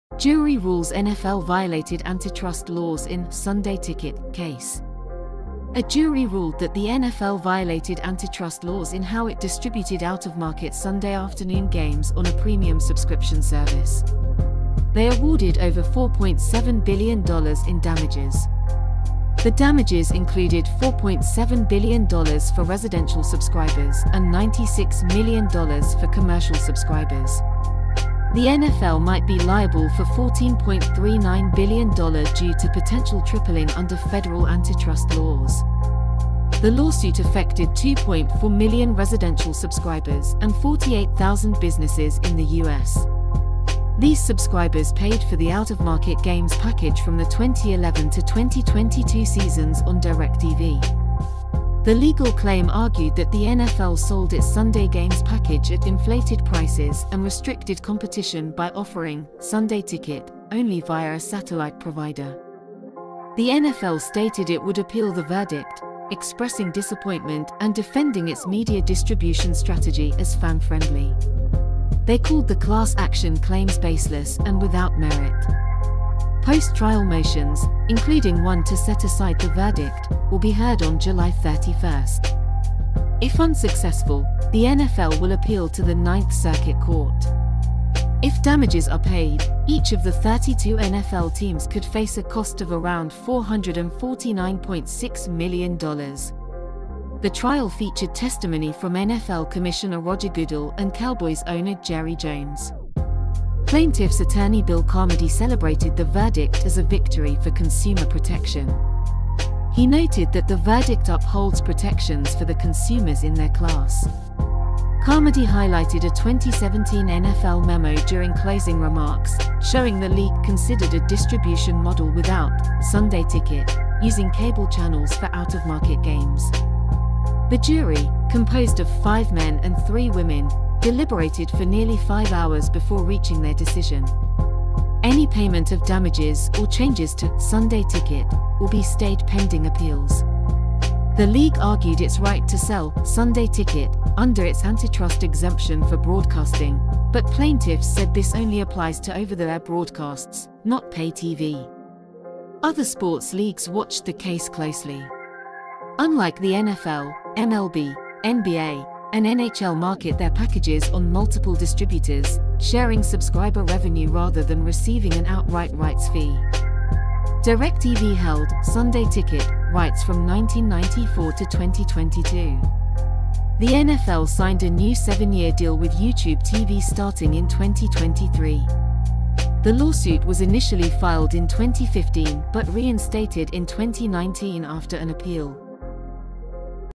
NFL Found GUILTY in 'Sunday Ticket' Case! Fans REJOICE with Victory
In a groundbreaking ruling, the jury has found the NFL guilty of violating antitrust laws in the 'Sunday Ticket' case, bringing a long-awaited victory to fans. The decision has sparked celebrations among football enthusiasts who have been eagerly awaiting justice in this high-profile legal battle.